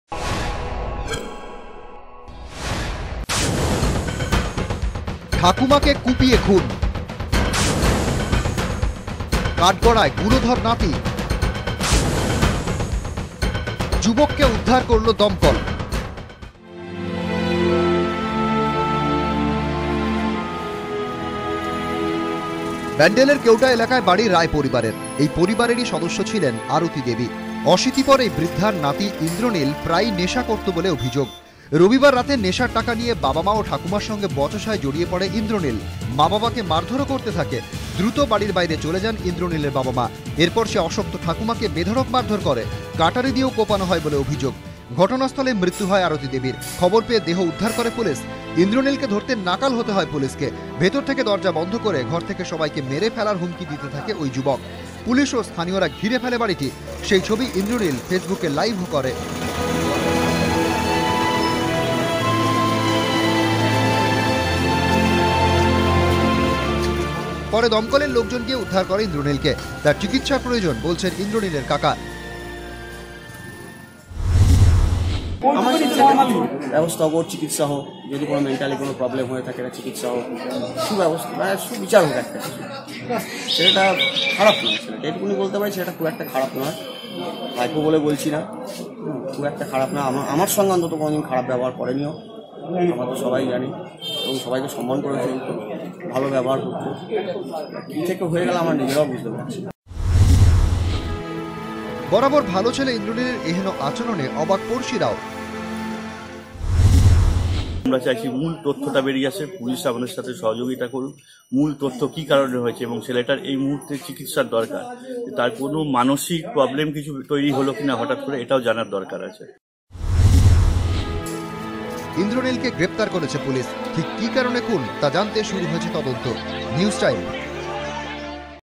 নাতির হাতে ঠাকুমা খুন || News Time Bangla
ব্যান্ডেলের কেওটা থানা এলাকায় এক পরিবারের এক যুবক, ইন্দ্রনীল রায় মদের নেশার জন্য টাকা নিয়ে বচসা হওয়ায় অশীতিপর ঠাকুমা কে কুপিয়ে খুন করে। পুলিশ তাকে গ্রেফতার করতে গেলে বাড়ির সকলকে খুন করার হুমকি দেয় ইন্দ্রনীল, এরপর দমকল এসে তাকে আটক করে। স্থানীয়দের মতে ইন্ড্রার মানসিক অবস্থা ঠিক নেই, তার চিকিৎসার প্রয়োজন।